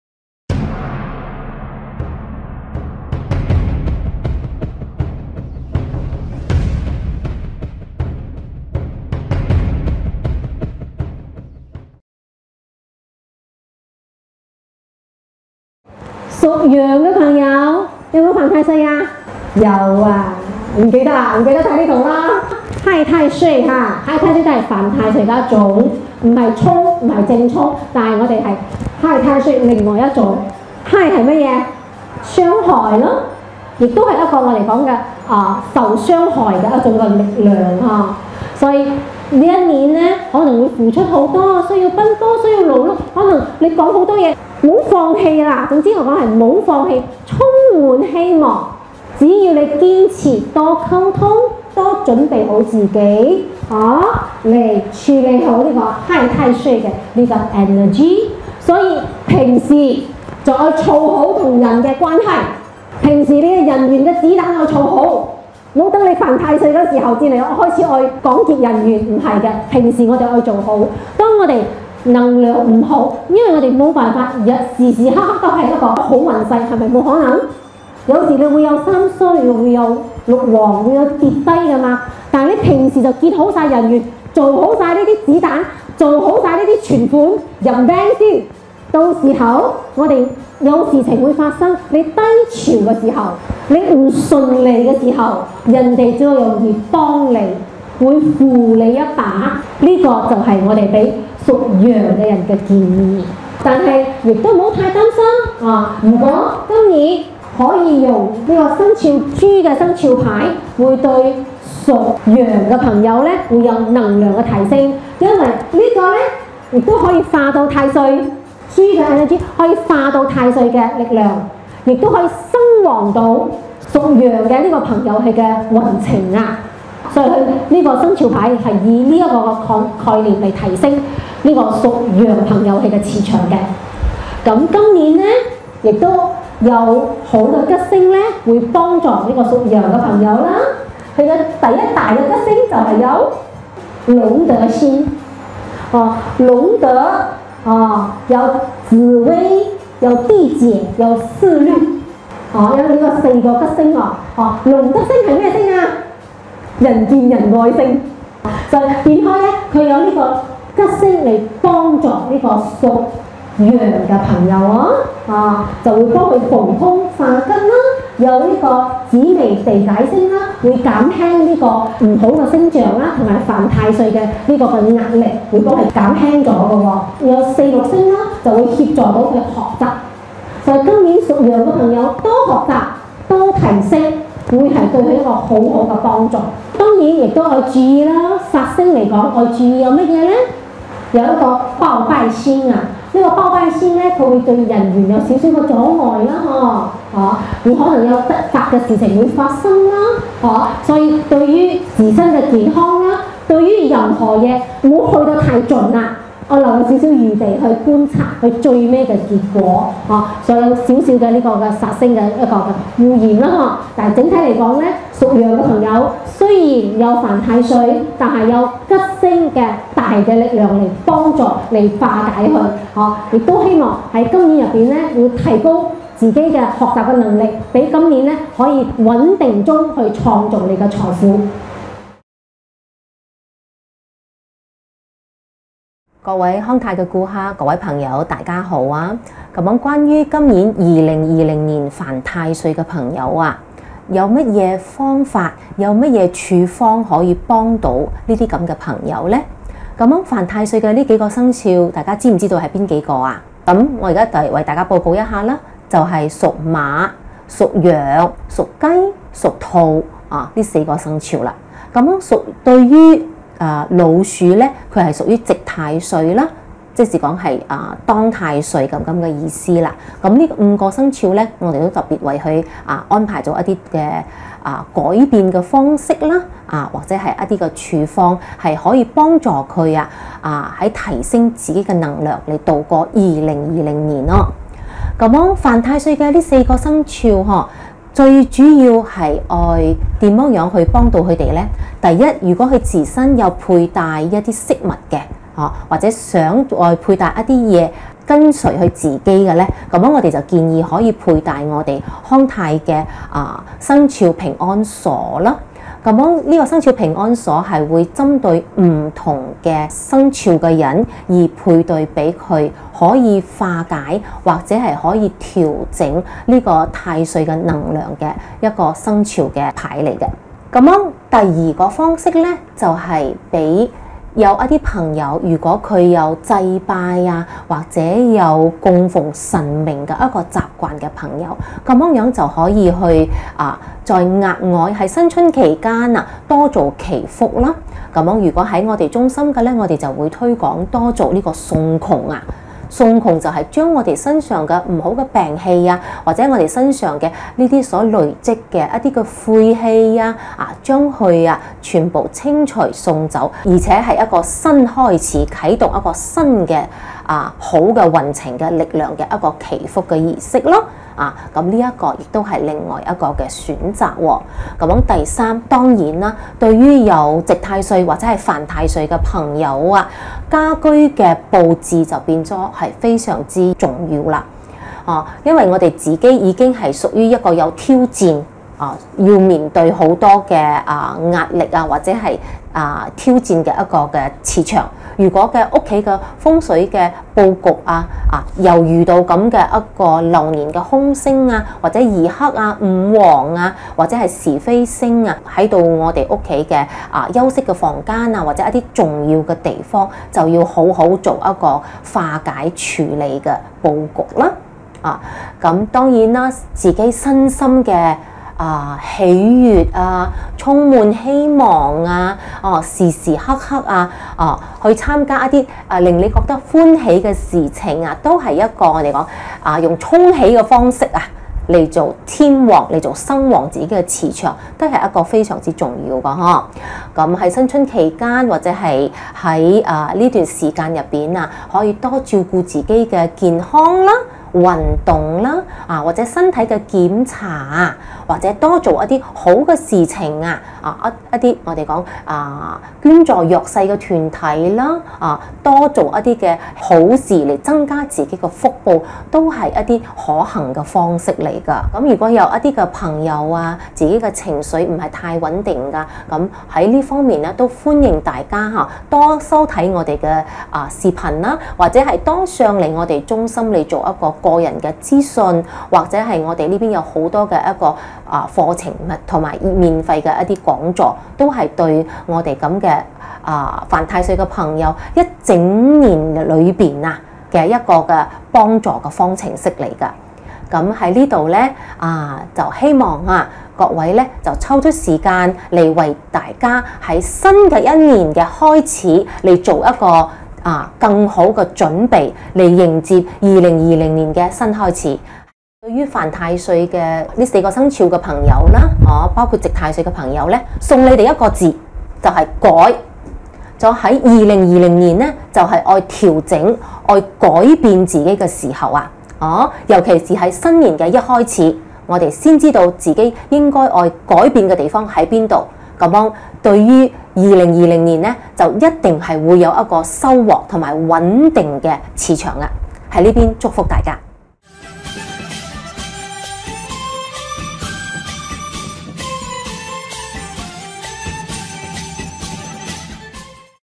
《2020吉鼠贺岁开运讲座》十二生肖运程—犯冲太岁的生肖？【羊】（粤）【康泰开运能量馆】
我2020年犯冲太岁哦！让康泰翁慧潔资深分析师为你分析今年的运势，还有“化解太岁之建议”。助你稳稳迎接2020年！

【康泰開運能量館】
有兴趣或任何疑问者欢迎联系: